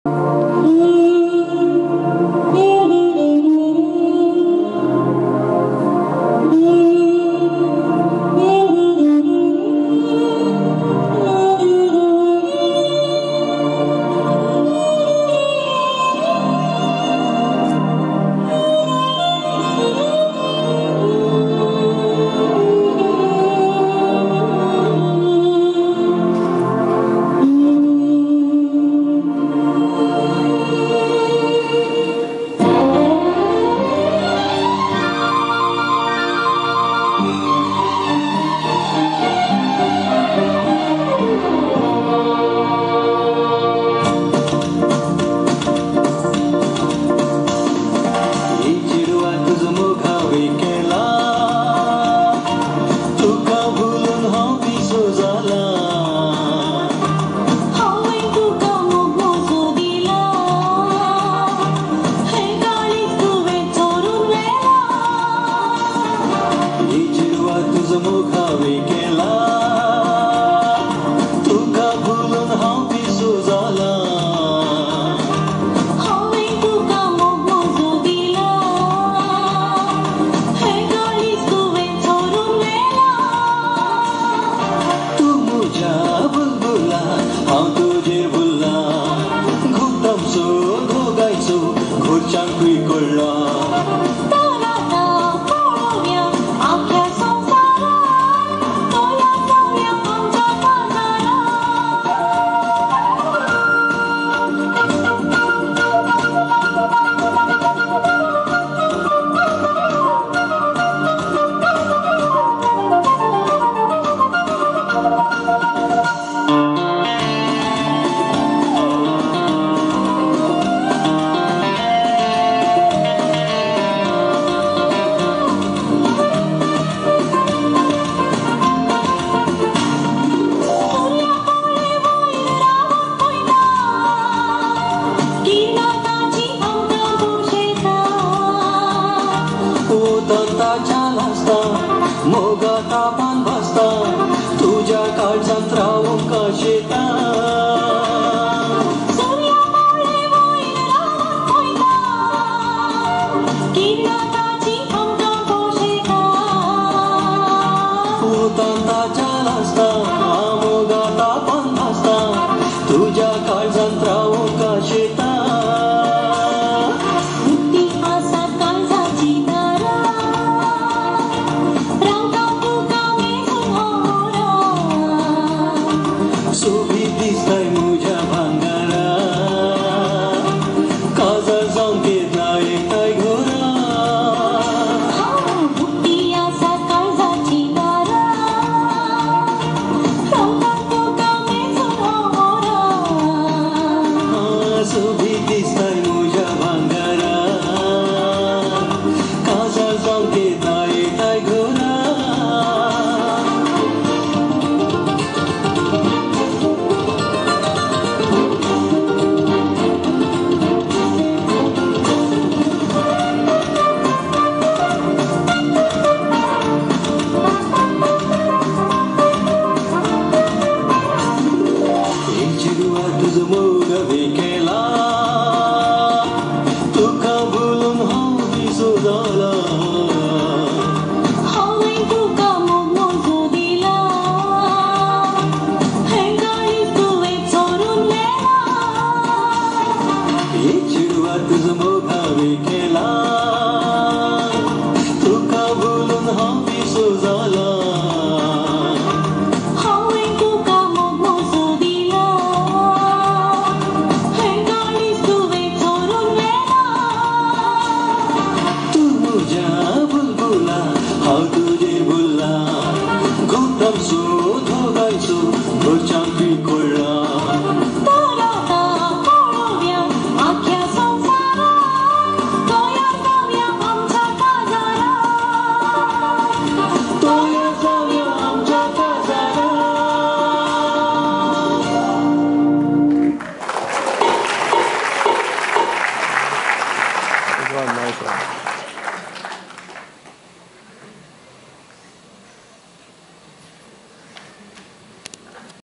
Konkani dance (to Baila music) ... Mangalore-style 2
Remona Pereira and Sandeep Sequeira, dance at the October 2023-end Sonvskrutayechim Daiz (Our Traditions in Culture) event organised by Michael Gracias and team at the Ravindra Bhavan, Margao, Goa. Recorded by Frederick Noronha (FN).